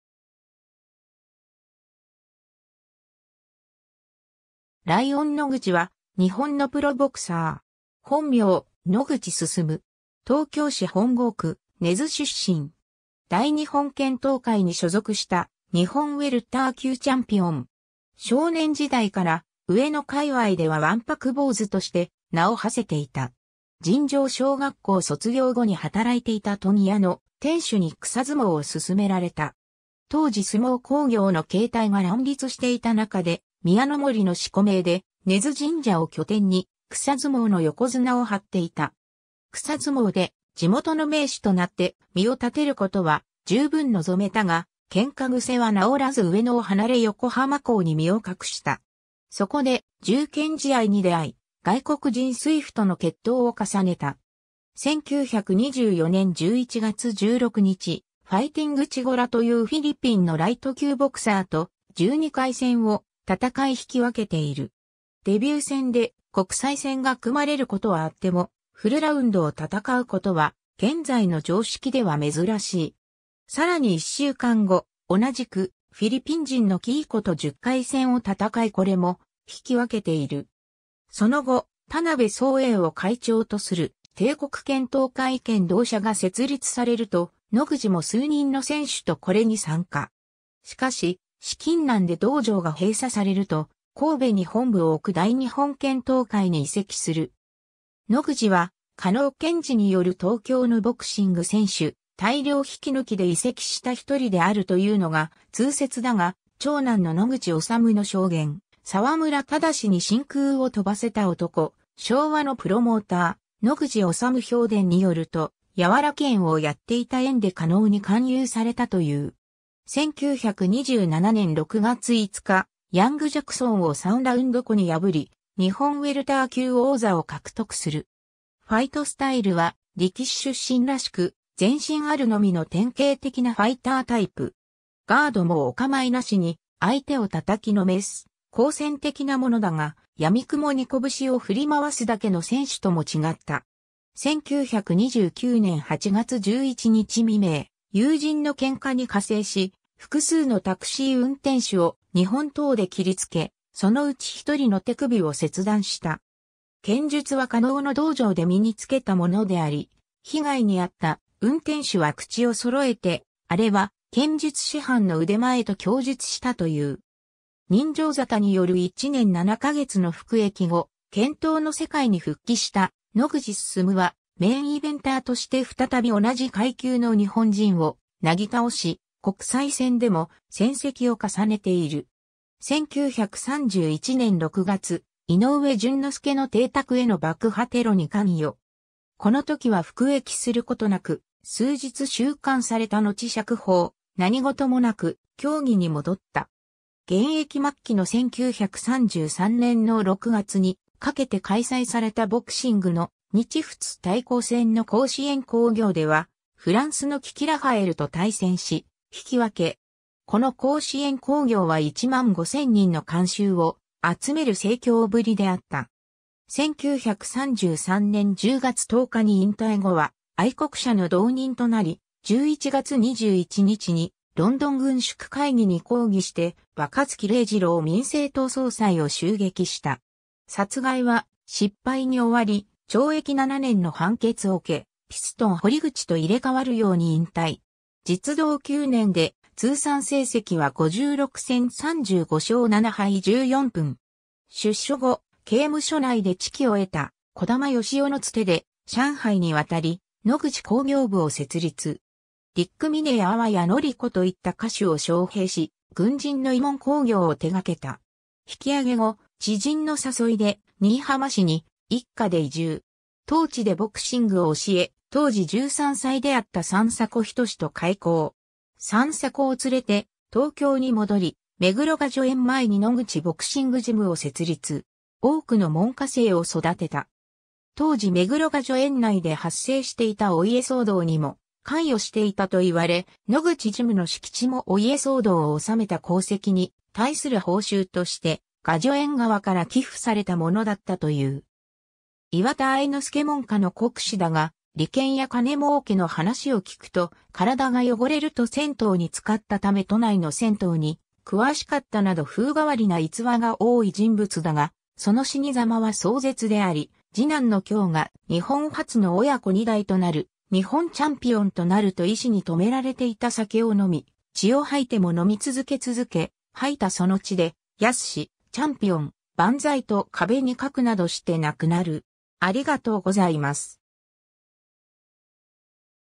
ライオン野口
ライオン野口（ライオンのぐち、男性　1908年6月2日 - 1961年5月8日、1907年生まれ説もある）は、日本のプロボクサー。
本名：野口 進（のぐち すすむ）。
東京市本郷区（現：東京都文京区）根津出身。
大日本拳闘会に所属した日本ウェルター級チャンピオン（1927年、1928年）。
 少年時代から上野界隈では腕白坊主として名を馳せていた。
尋常小学校卒業後に働いていた問屋の店主に草相撲を勧められた。
当時相撲興業の形態が乱立していた中で、「宮の森」の四股名で根津神社を拠点に草相撲の横綱を張っていた。
 草相撲で地元の名士となって身を立てることは十分臨めたが、喧嘩癖は治らず上野を離れ横浜港に身を隠した。
そこで「柔拳試合」に出会い、外国人水夫との決闘を重ねた。
 1924年11月16日、ファイティング・チゴラというフィリピンのライト級ボクサーと、12回戦を戦い引き分けている。
デビュー戦で国際戦が組まれることはあっても、フルラウンド（12回戦）を戦うことは現在の常識では珍しい。
さらに1週間後、同じくフィリピン人のキーコと10回戦を戦いこれも引き分けている。
 その後、田辺宗英を会長とする帝国拳闘会拳道社（現・帝拳ボクシングジム）が設立されると、野口も数人の選手とこれに参加。
しかし、資金難で道場が閉鎖されると、神戸に本部を置く大日本拳闘会（大日拳）に移籍する。
野口は嘉納健治による東京のボクシング選手大量引き抜きで移籍した1人であるというのが通説だが、長男の野口修の証言『沢村忠に真空を飛ばせた男／昭和のプロモーター・野口修評伝』によると、柔拳をやっていた縁で嘉納に勧誘されたという。
 1927年6月5日、ヤング・ジャクソン（アメリカ）を3ラウンドKOに破り、日本ウェルター級王座を獲得する。
 ファイトスタイルは力士出身らしく、前進あるのみの典型的なファイタータイプ。
ガードもお構いなしに、相手を叩きのめす好戦的なものだが、闇雲に拳を振り回すだけの選手とも違った。
 1929年8月11日未明、友人の喧嘩に加勢し、複数のタクシー運転手を日本刀で斬りつけ、その内1人の手首を切断した。
剣術は嘉納の道場で身につけたものであり、被害に遭った運転手は口を揃えて「あれは剣術師範の腕前」と供述したという。
 刃傷沙汰による1年7ヶ月の服役後、拳闘の世界に復帰した野口進は、メインイベンターとして再び同じ階級の日本人をなぎ倒し、国際戦でも戦績を重ねている。
 1931年6月、井上準之助の邸宅への爆破テロに関与。
このときは服役することなく、数日収監されたのち釈放、何事もなく競技に戻った。
 現役末期の1933年の6月にかけて開催されたボクシングの「日仏対抗戦」の甲子園興行ではフランスのキキ・ラファエルと対戦し、引き分け。
この甲子園興行は1万5千人の観衆を集める盛況ぶりであった。
 1933年（昭和8年）10月10日に引退後は愛国社の同人となり、11月21日にロンドン軍縮会議に抗議して若槻禮次郎民政党総裁を襲撃した。
殺害は失敗に終わり、懲役7年（控訴審で5年に減刑）の判決を受け、ピストン堀口と入れ替わるように引退。
実働9年で通算成績は56戦35勝7敗14分。
 出所後、刑務所内で知己を得た児玉誉士夫の伝手で上海に渡り、野口興行部を設立。
ディック・ミネや淡谷のり子といった歌手を招聘し、軍人の慰問興行を手掛けた。
 引き揚げ後、知人の誘いで新居浜市に一家で移住。
当地でボクシングを教え、当時13歳であった三迫仁志と邂逅。
三迫を連れて東京に戻り、目黒雅叙園前に野口ボクシングジム（目黒ジムと呼ぶ）を設立。
多くの門下生を育てた。
当時目黒雅叙園内で発生していたお家騒動（創業家である細川家の相続争い）にも関与していたと言われ、野口ジムの敷地もお家騒動を収めた功績に対する報酬として雅叙園側から寄付されたものだったという。
 岩田愛之助門下の国士だが、利権や金儲けの話を聞くと「身体が汚れる」と銭湯に漬かったため都内の銭湯に詳しかったなど風変わりな逸話が多い人物だが、その死に様は壮絶であり、次男の恭が日本初の親子二代となる日本チャンピオンとなると医師に止められていた酒を飲み、血を吐いても飲み続け続け、吐いたその血で「恭、チャンピオン、万歳」と壁に書くなどして亡くなる。